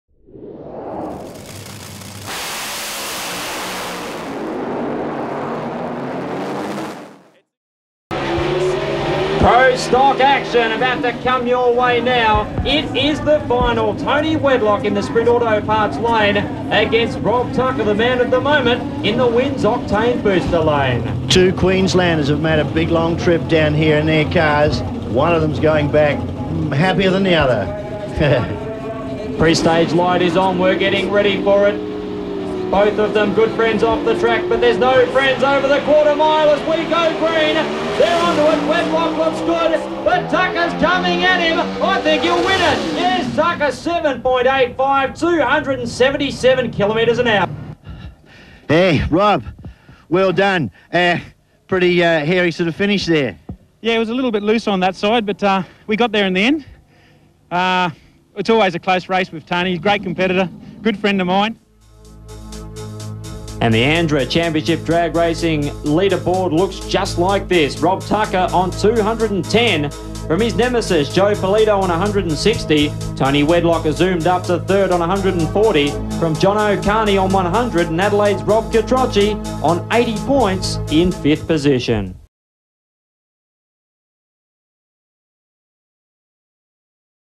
ThrowbackThursday - Pro Stock - Adelaide International Raceway 1997
ANDRA ThrowbackThursday - Rob Tucker takes on Tony Wedlock in the final of Round 2 of the 1997 Australian Pro Stock Championship at Adelaide International Raceway